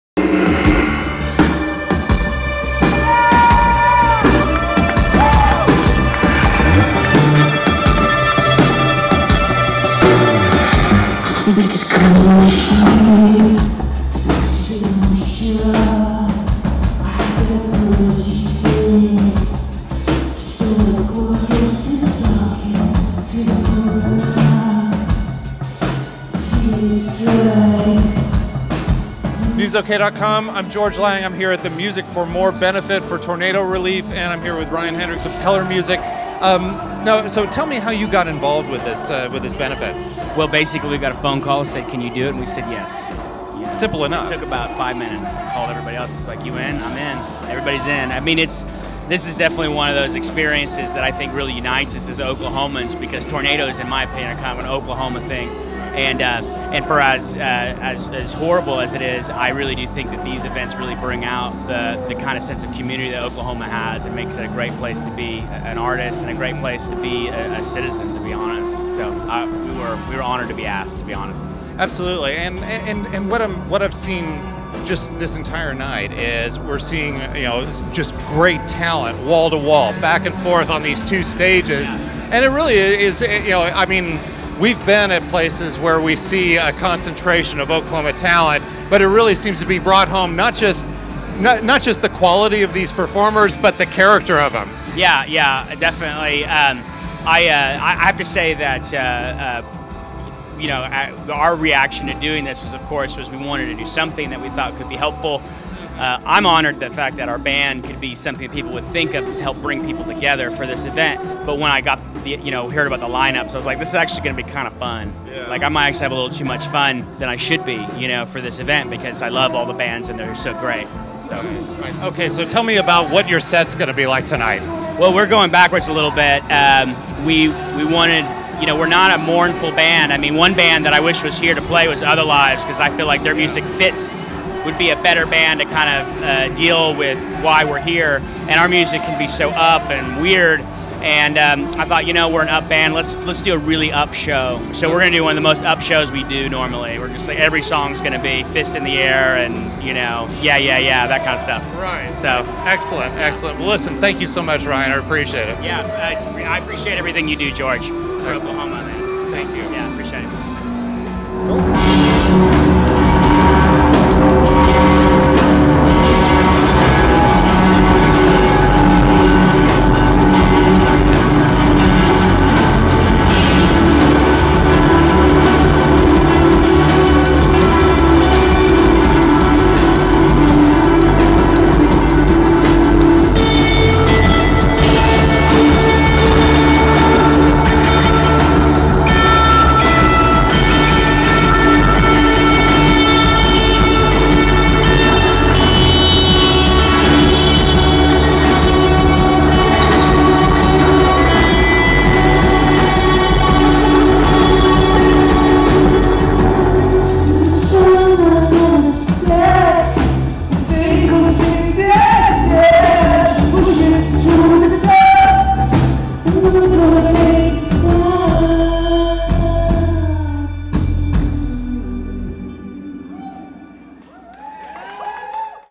Music4Moore Ryan Hendrix of Colourmusic (2013-05-30)
George Lang talks with Ryan Hendrix from Colourmusic during the Music 4 Moore benefit concert.